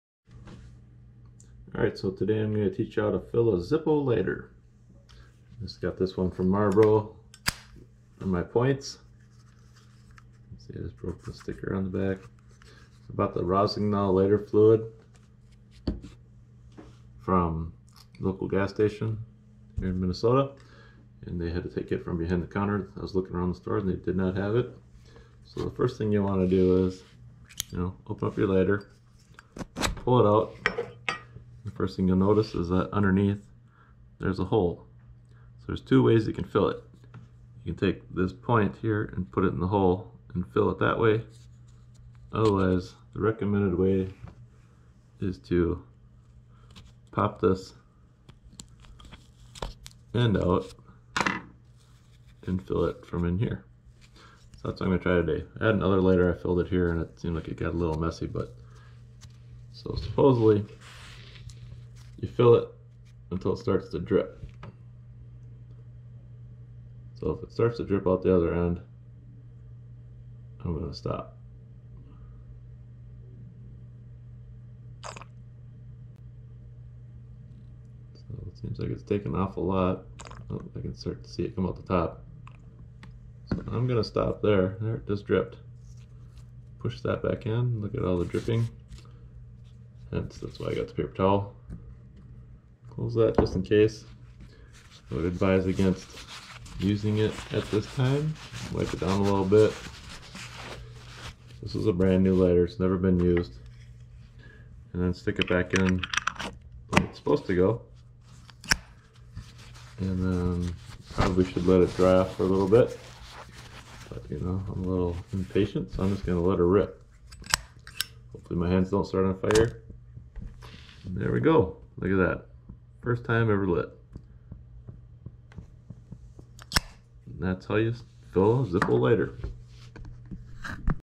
Zippo Ronsonol Lighter Fuel FIll
How to fill a brand new Marlboro points Zippo lighter with Ronsonol lighter fuel.

Ronsonol Lighter Fuel 12 fl oz (148 ml) ($7.59) :

Zippo - 4 oz. ($3.50) :